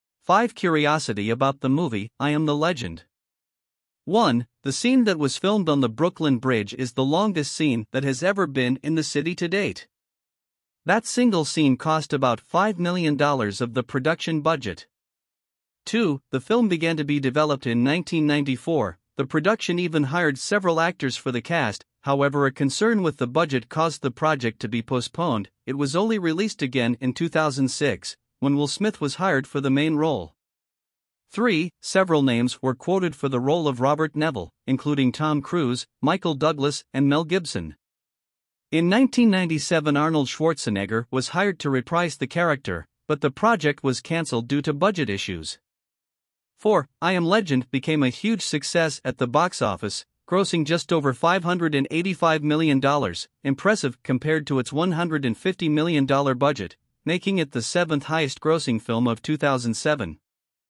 4 CURIOSITY ABOUT THE MOVIE I AM THE LEGEND ‐ Feito com o Clipchamp
Social curiosity is talking, listening and watching others to learn what they are thinking and doing. “Human beings are inherently social animals, and the most effective and efficient way to determine whether someone is friend or foe is to obtain information.”

4 CURIOSITY ABOUT THE MOVIE I AM THE LEGEND

1, The scene that was filmed on the Brooklyn Bridge is the longest scene that has ever been in the city to date. That single scene cost about $5 million of the production budget.
2, The film began to be developed in 1994, the production even hired several actors for the cast, however a concern with the budget caused the project to be postponed, it was only released again in 2006, when Will Smith was hired for the main role.
3, Several names were quoted for the role of Robert Neville, including Tom Cruise, Michael Douglas and Mel Gibson. In 1997 Arnold Schwarzenegger was hired to reprise the character, but the project was canceled due to BUDGET issues.
4, I Am Legend became a huge success at the box office, grossing just over $585 million, impressive compared to its $150 million budget, making it the seventh highest-grossing film of 2007.